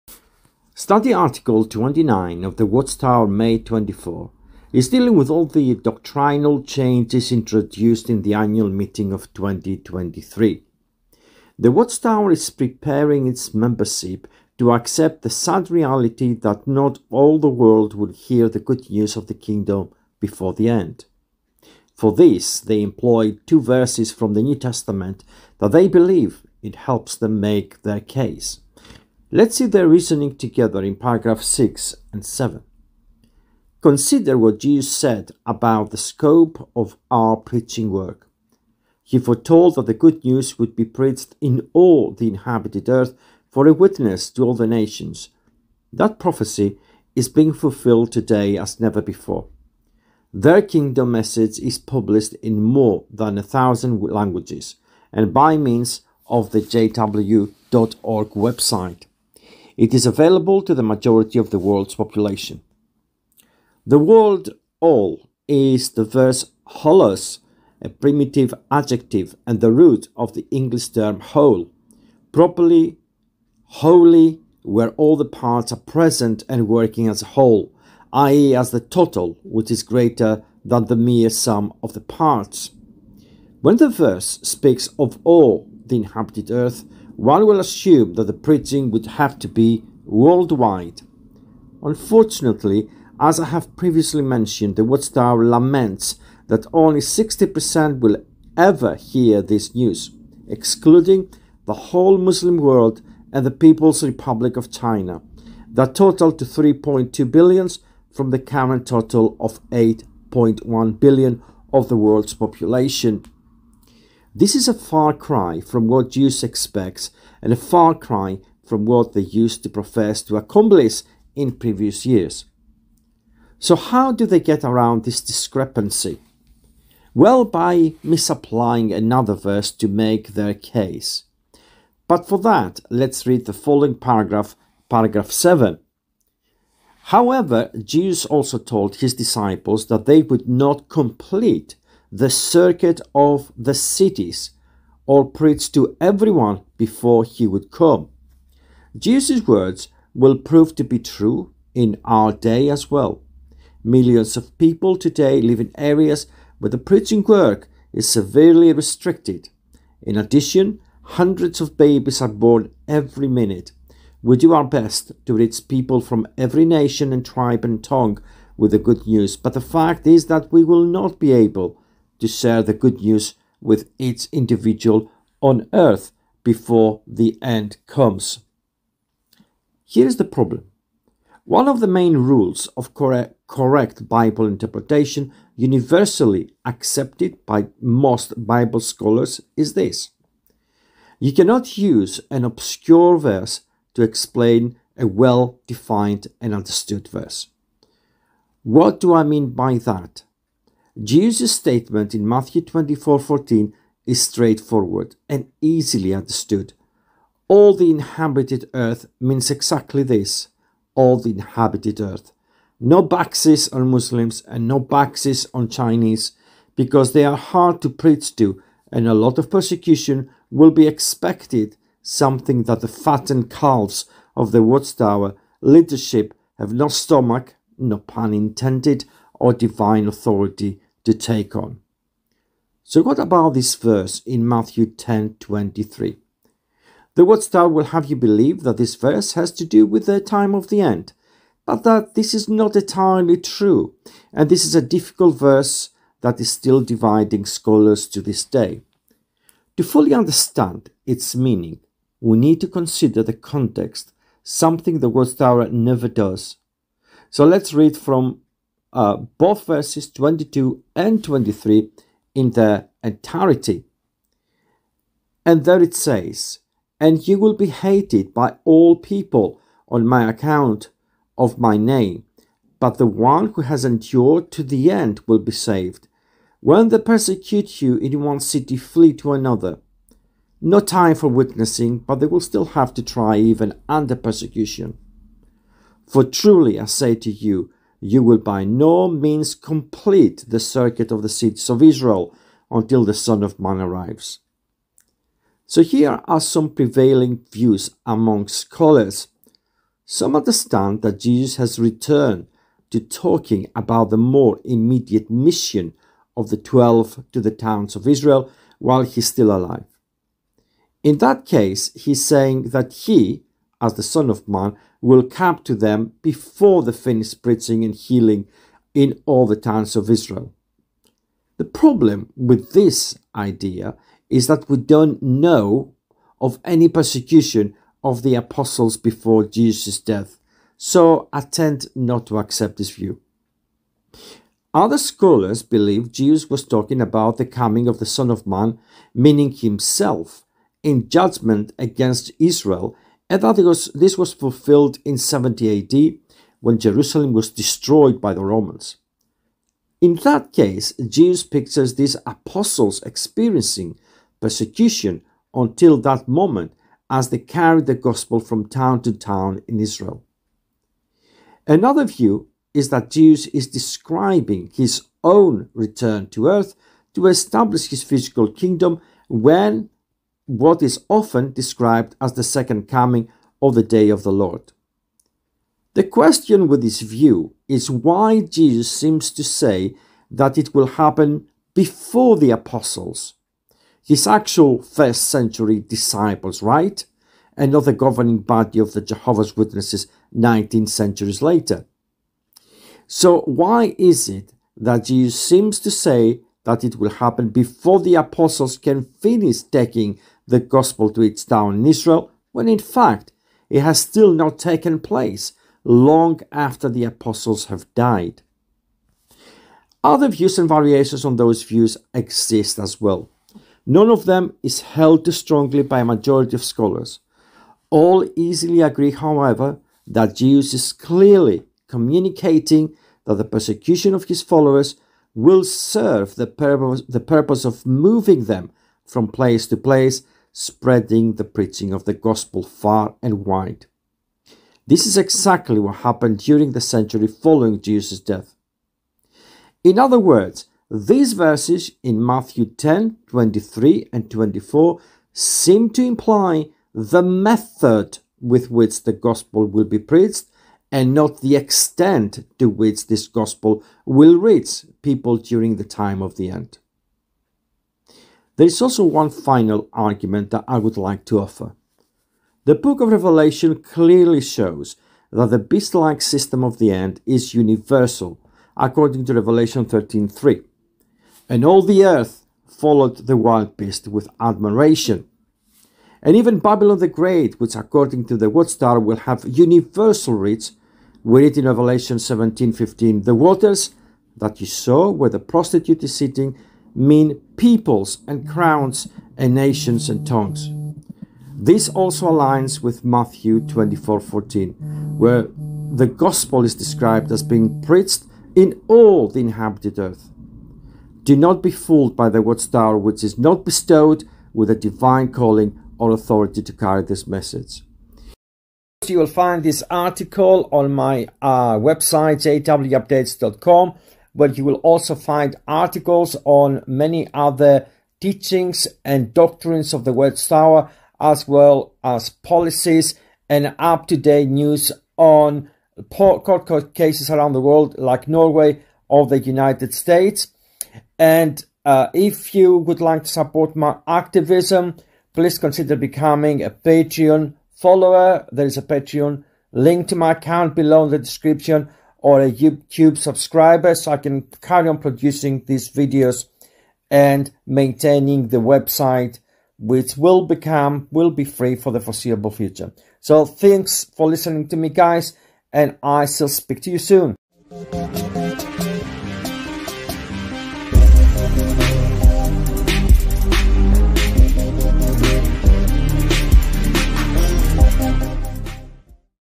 NEW LIGHT: The CIRCUIT of the CITIES of ISRAEL
Study Article 19 of the Watchtower May '24 is dealing with all the doctrinal changes introduced in the Annual Meeting of 2023. The Watchtower is preparing its membership th accept the sad reality that NOT all the world will hear the good news of the Kingdom before the End. For this they employ two verses from the New Testament that they believe it helps them make their case.